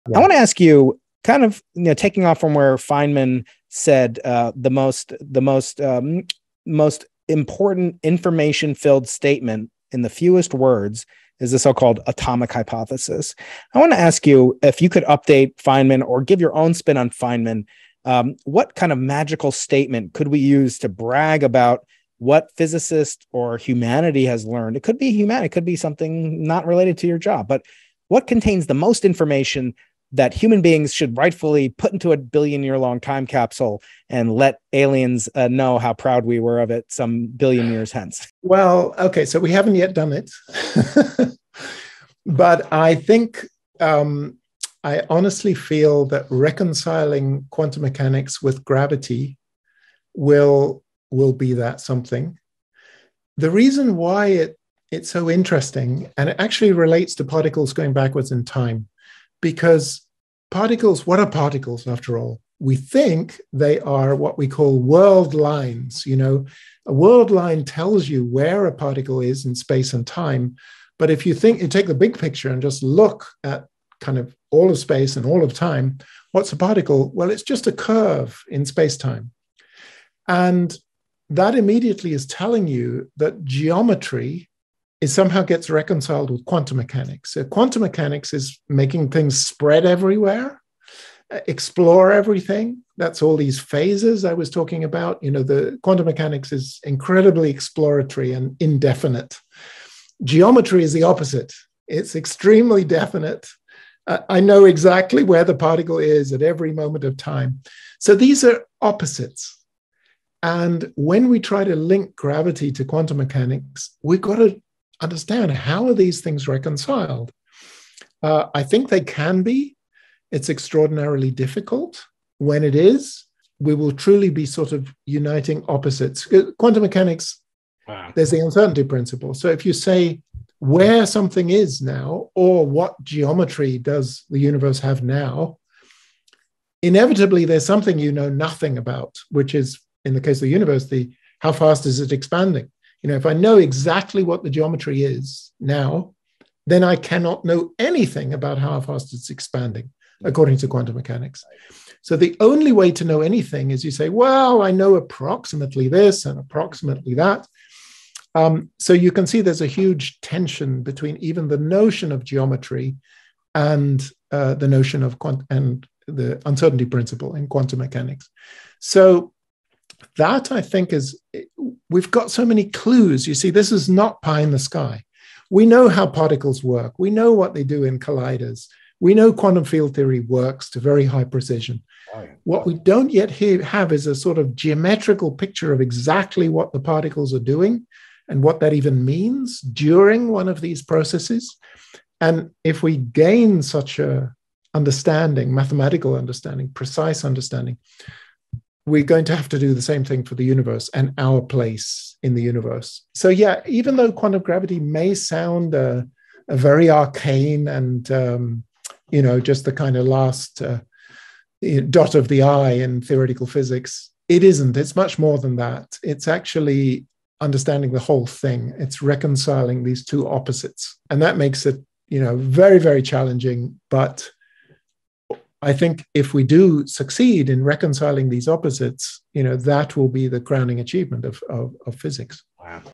What Would Neil Turok Put In a Time Capsule for Aliens to Find?
to win a real 4 billion year old meteorite! All .edu emails in the USA 🇺🇸 will WIN!

Which theory does Neil Turok think is so fundamental that he would put it in a time capsule for aliens to find and learn about humans? Find out in this clip from our previous interview!

00:00 Introduction
01:00 The time capsule
03:00 the future